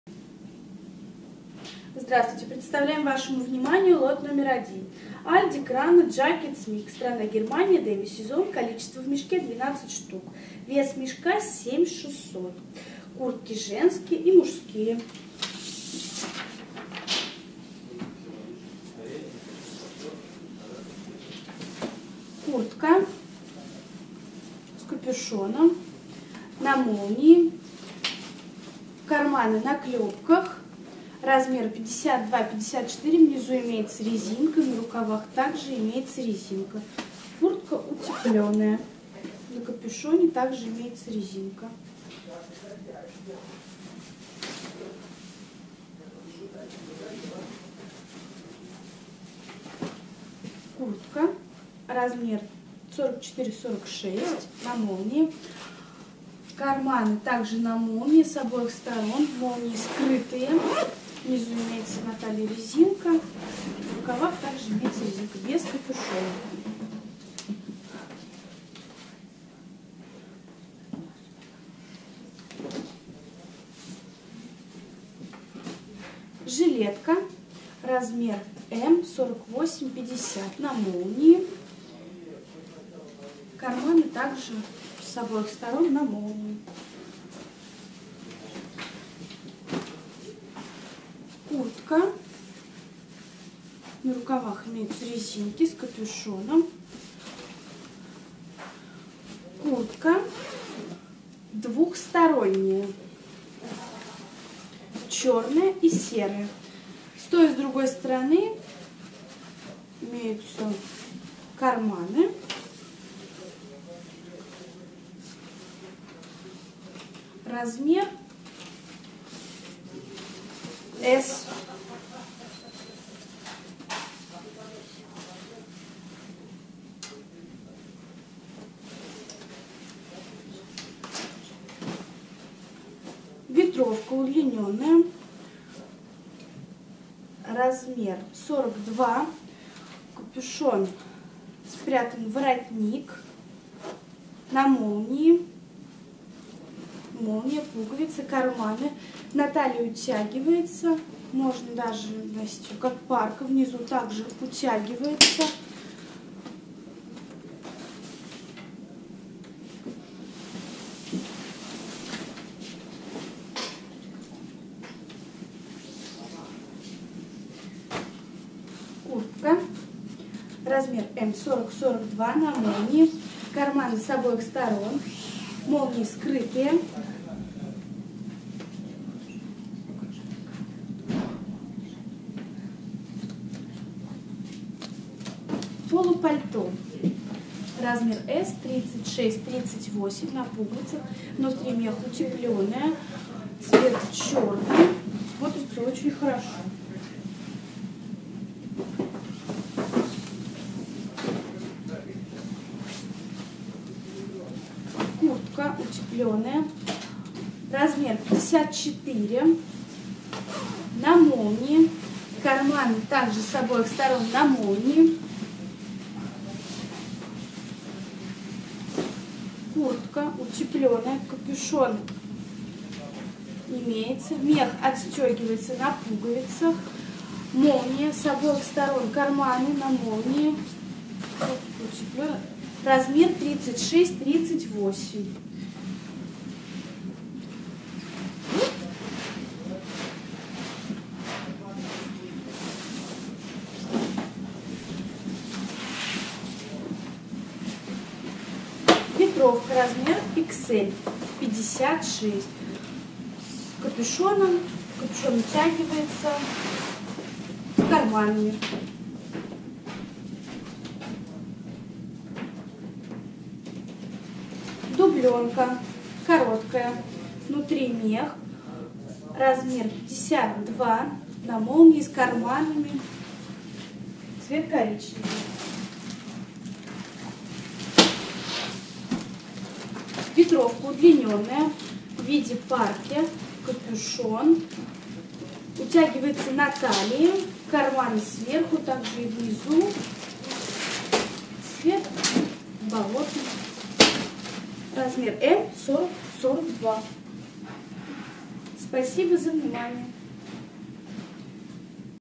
ALDI CRANE JACKETS MIX 1, Цена за кг: 16 евро, сток одежда оптом
ALDI (CRANE) JACKETS MIX ( Лот № 1)
Куртки женские и мужские  (сток)
Страна: Германия
Сезон: Демисезон
Цена за кг.: 16  евро
Цена за мешок: 121.6 евро
Цена за ед.: 10.13 евро